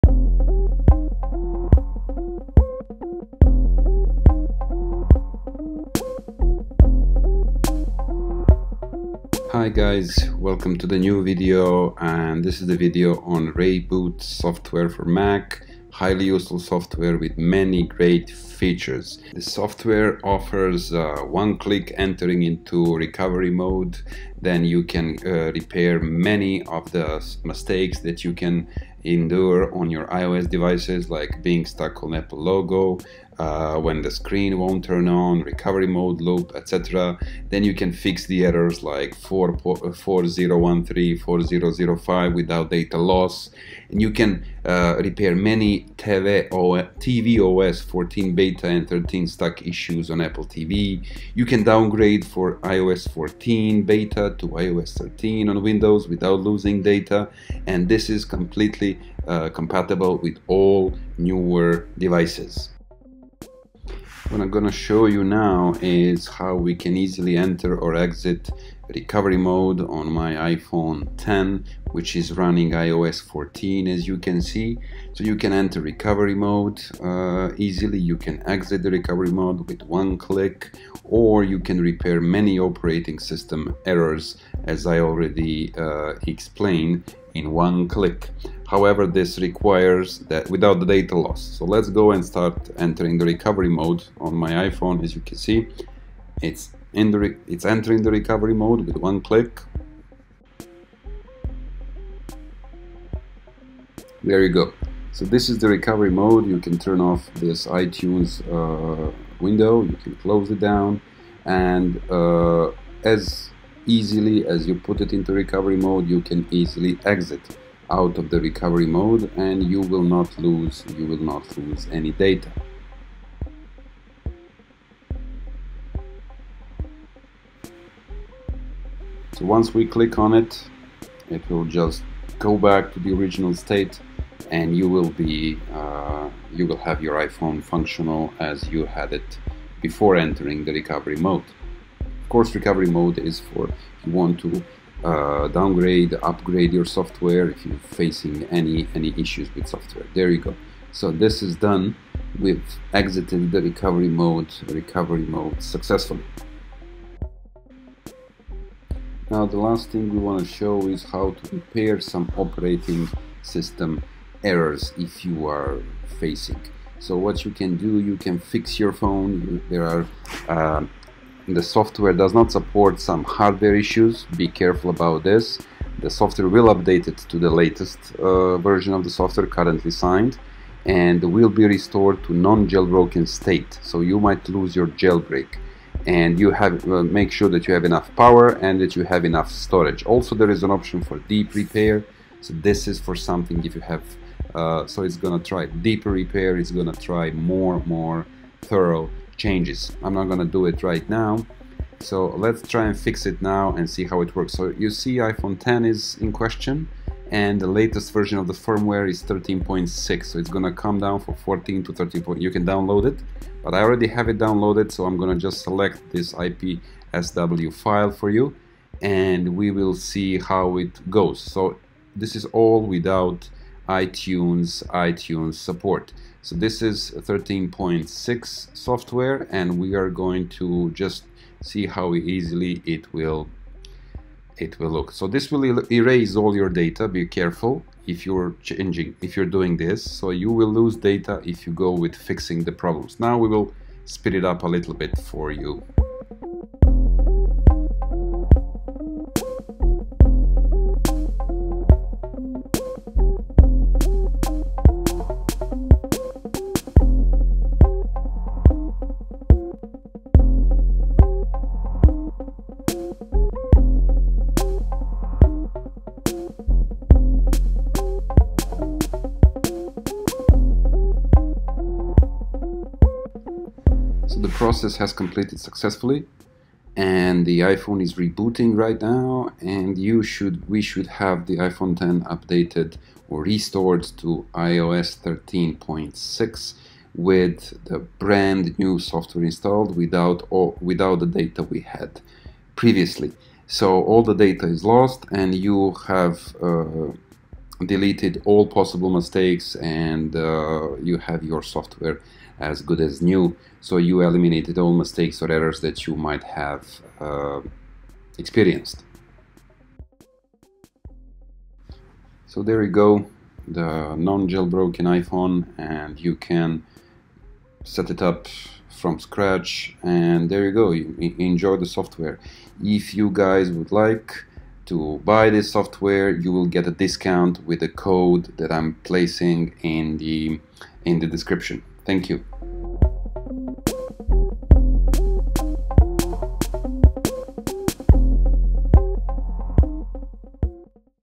How to Fix Any iOS System Issue Without Data Loss - Stuck at Recovery Mode/White/Black Screen & More
In the video I am presenting great software for quick resolution of various iOS problems you might encounter, like stuck at Recovery Mode/White Screen/Black Screen & Many More (iPhone, iPad , iPod).

Tenorshare ReiBoot (Mac) - World's No.1 iPhone Recovery Mode Tool (FREE) & iOS 14 Beta System Recovery Software.